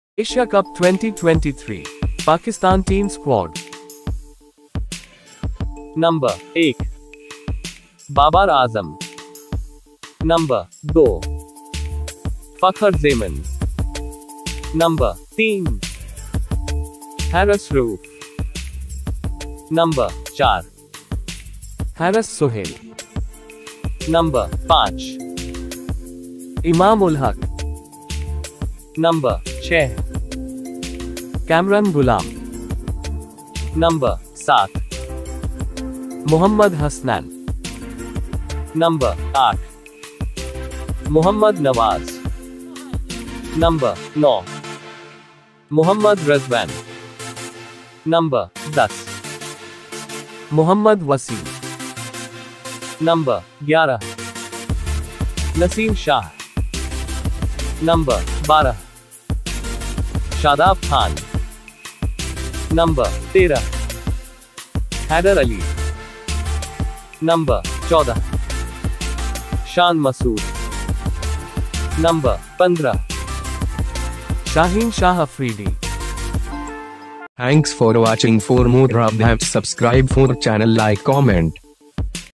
Asia Cup Cricket 2023 Pakistan Team Squad 2023 Pakistan Squad Asia Cup 2023
pakistan squad
1. Babar Azam (capt) 2. Fakhar Zaman 3. Haris Rauf 4. Haris Sohail 5.Imam-ul-Haq 6. Kamran Ghulam 7. Mohammad Hasnain 8. Mohammad Nawaz 9.Mohammad Rizwan (wk), 10. Mohammad Wasim 11. Naseem Shah 12. Shadab khan 13. Haidar Al 14. Shan Masood 15. Shaheen Shah Afridi